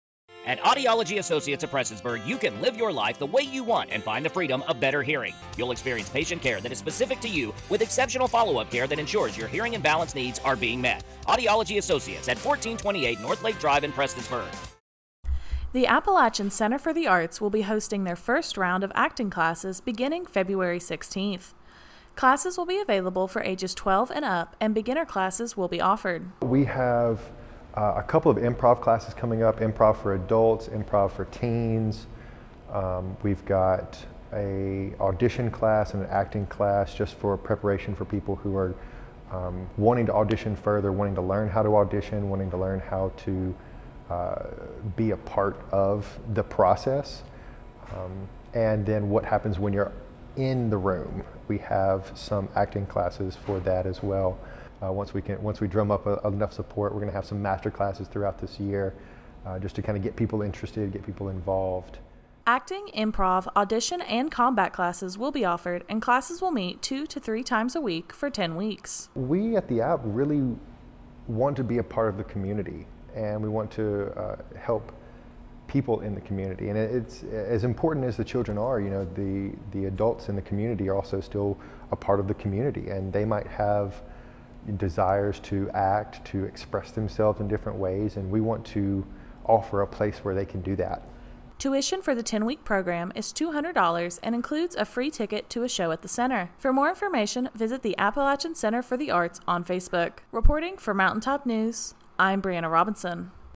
Acting Classes Starting February 16 at the App
The Appalachian Center for the Arts will begin their first round of acting classes for the year starting February 16.  Classes begin for kids age 12 and up as well as separate classes for adults.